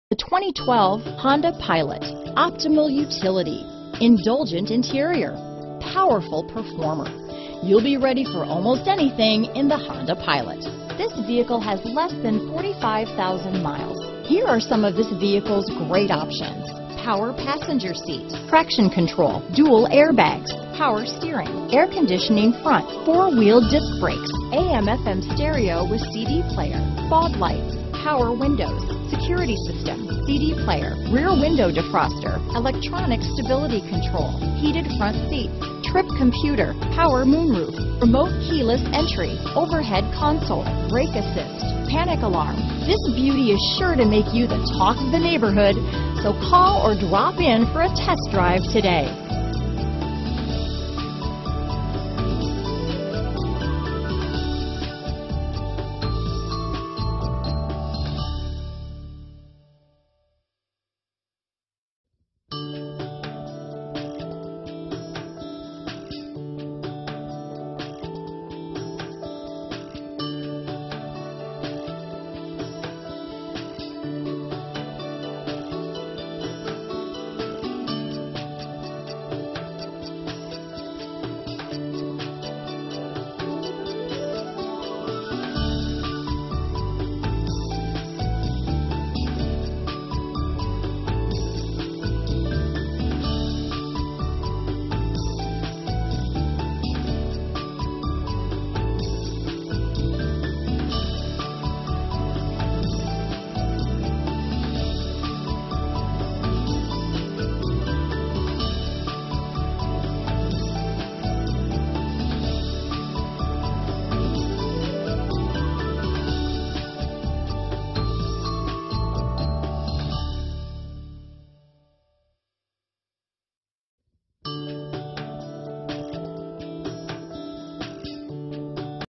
2012 Honda Pilot Schaumburg, Arlington Heights, Elgin, Naperville, Lisle, IL 80745A
Schaumburg Honda
750 E Golf Rd

**New Arrival** **Honda Certification Included In The Price** **Ex-L 4WD Model** **Good Mileage For The Year** This Pilot Is Equipped With: Four Wheel Drive, 3.5L V6 Engine, Power Tailgate, Usb Audio Jack, Xm Radio Capability, Tow Package, 8 Passenger Seating, Rear Backup Camera, Power Sunroof, Heated Front Seats, Fog Lights, Automatic Climate Control, Abs Brakes, Keyless Remote Entry, Cd Player, Alloy Wheels, Fold Down Rear Seats, Privacy Glass, Cruise Control And Much More. This Honda Pilot Ex-L 4WD Won't Last Long.